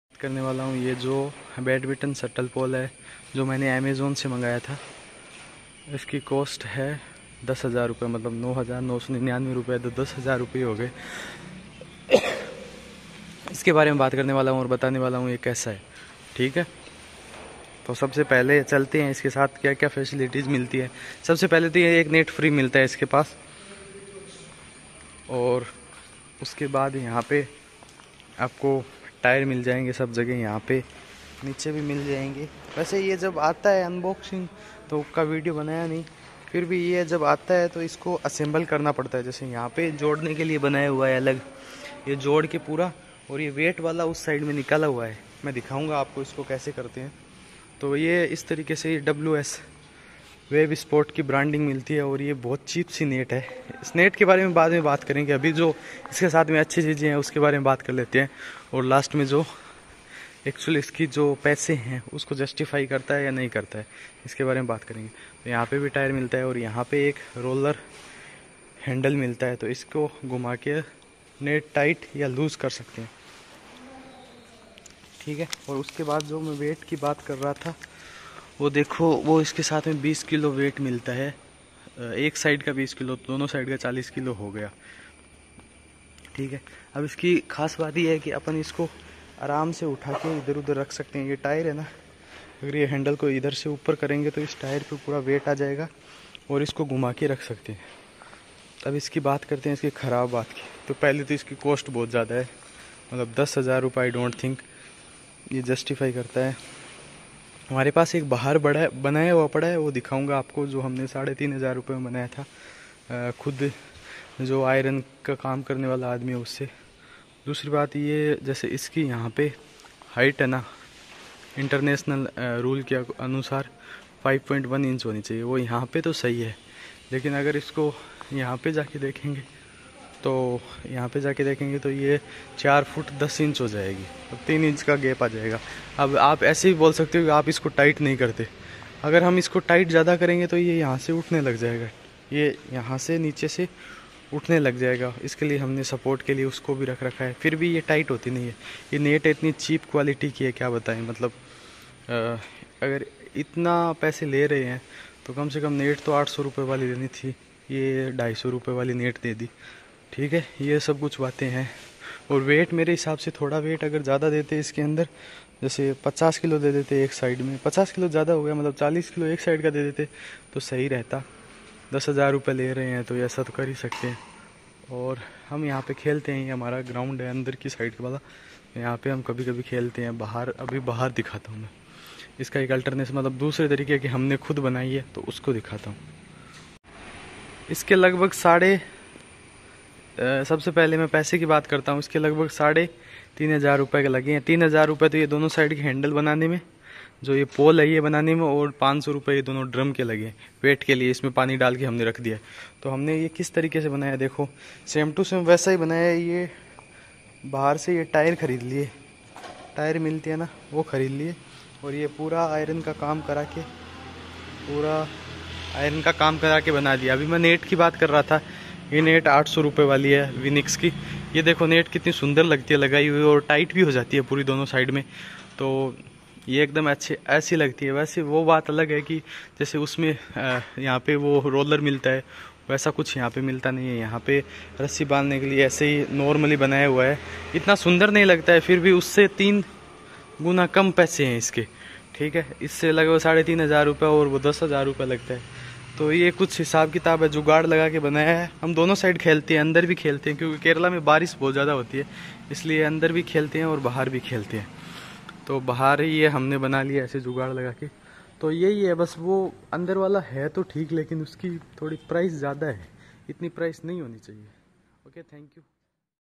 Badminton Pole Full Review in Hindi | WEB SPORTS WS Badminton Pole, Movable, Family Model
In this video I am review WEB SPORTS WS Badminton Pole which i purchased from amazon and cost is 9999 this is movable badminton pole easy to move and good look

Badminton Pole Full Review  in Hindi
shuttle court pole review
movable badminton pole
badminton pole
badminton court review
badminton court